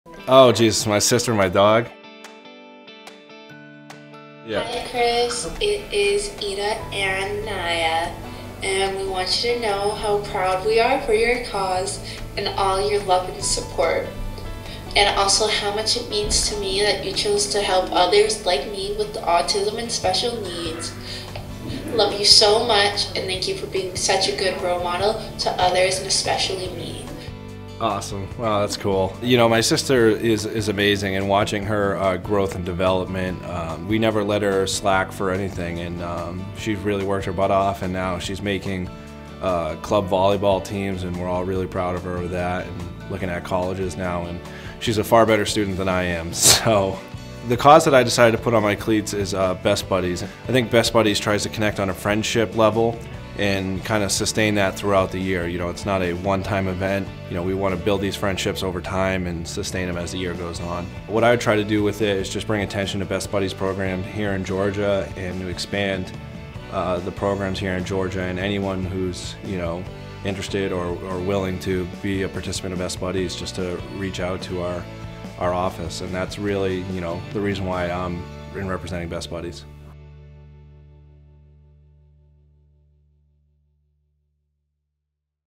Chris Lindstrom represents Best Buddies for 2021 My Cause My Cleats | Atlanta Falcons | NFL
Atlanta Falcons Guard Chris Lindstrom represents Best Buddies for 2021 My Cause My Cleats game against the Tampa Bay Buccaneers at Mercedes-Benz Stadium.